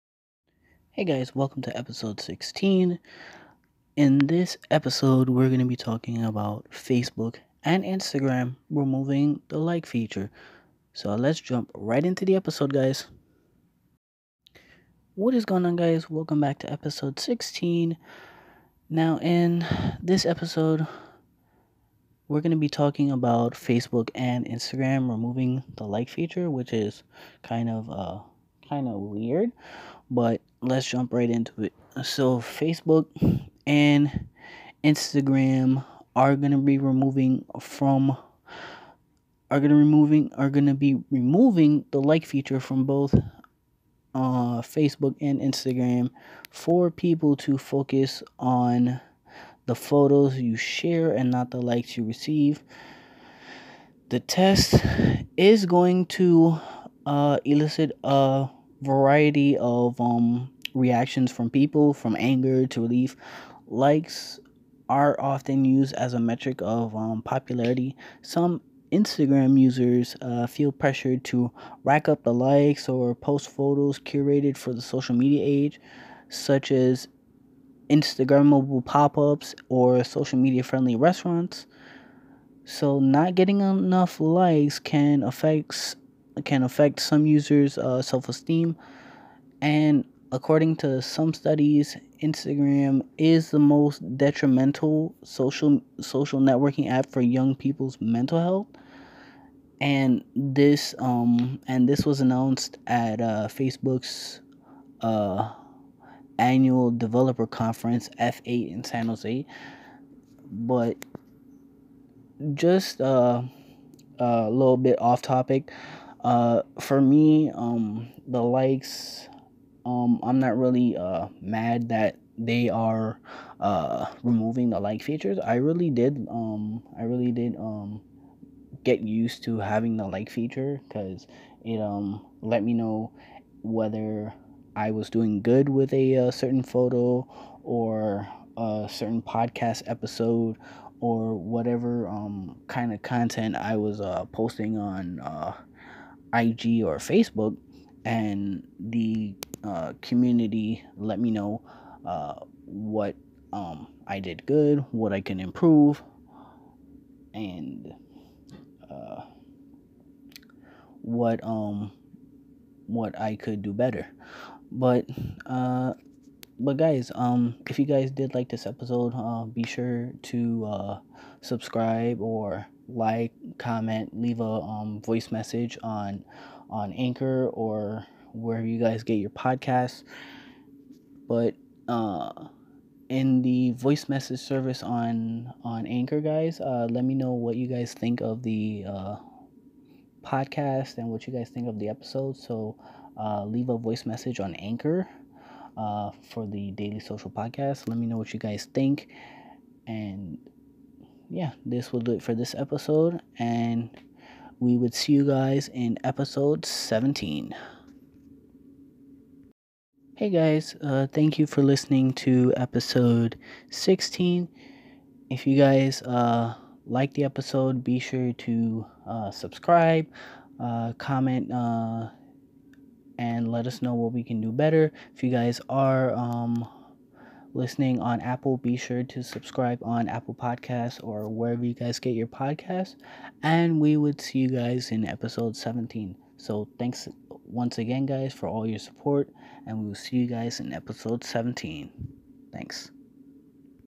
Facebook & IG removing likes
Facebook & Instagram are going to be removing the like feature from IG and FB for people to focus on the photos you share and not the likes you receive.

The test is likely to elicit a range of reactions, from anger to relief. Likes are often used as a metric of popularity. Some Instagram users feel pressure to rack up the likes or post photos curated for the social media age, such as at Instagrammable pop-ups or social-media friendly restaurants. Not getting enough likes can even affect some users' self-esteem. According to some studies, Instagram is the most detrimental social networking app for young people's mental health.

The news was announced at Facebook's (FB) annual developer conference, F8, in San Jose.